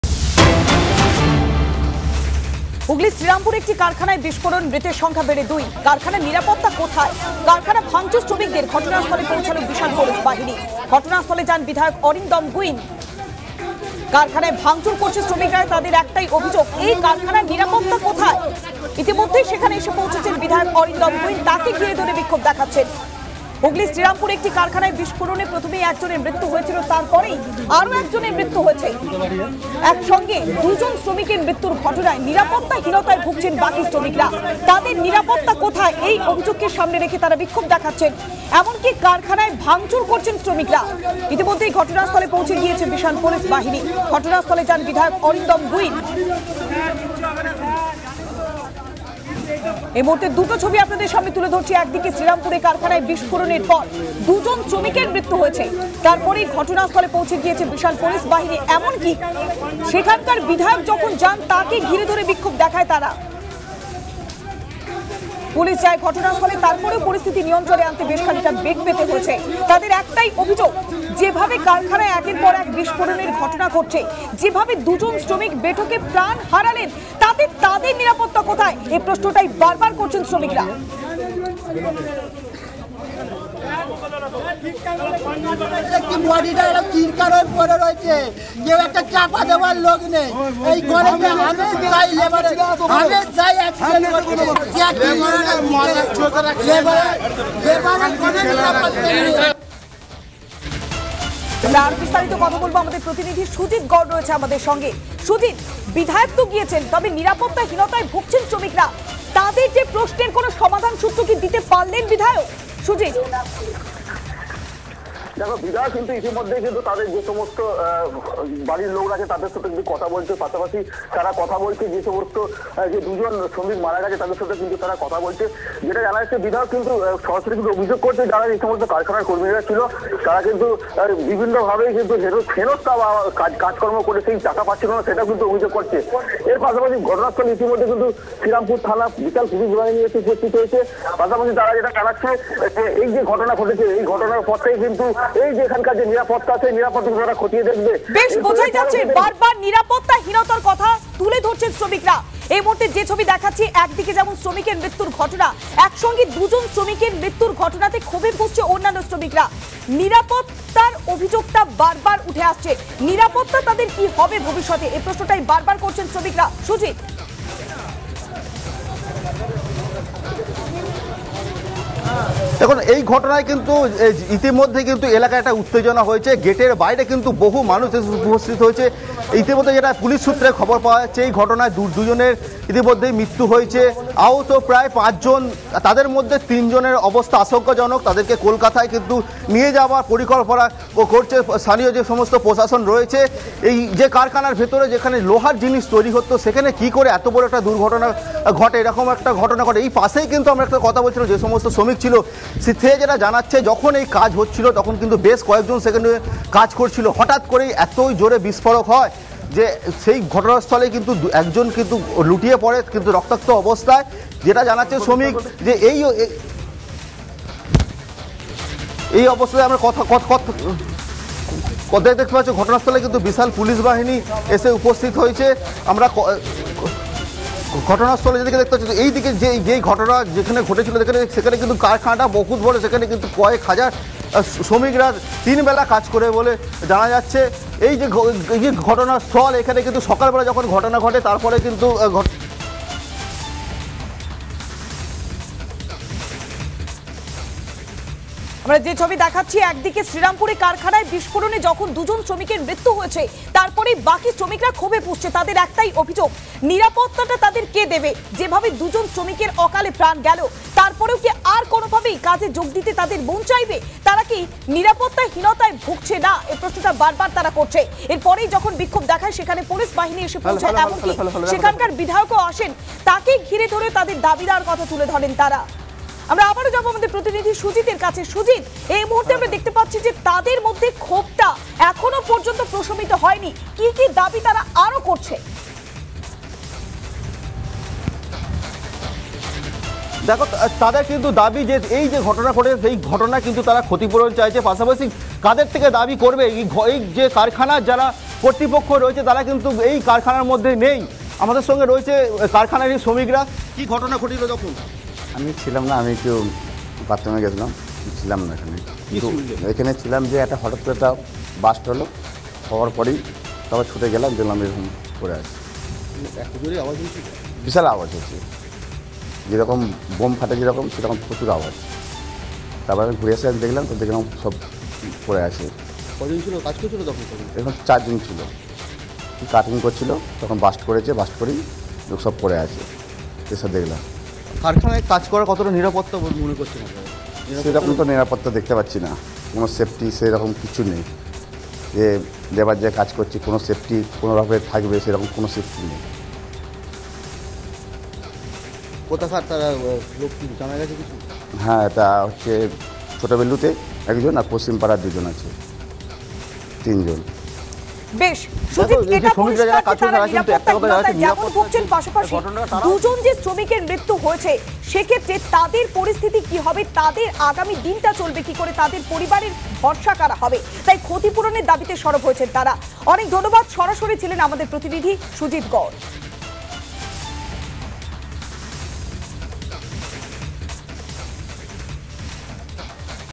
Sreerampore News | শ্রীরামপুরে কারখানায় গ্যাস সিলিন্ডার ফেটে মৃত ১। আশঙ্কাজনক আরও ১
রিপাবলিক স্বাধীন।
রিপাবলিক নির্ভীক।
আপনিই রিপাবলিক।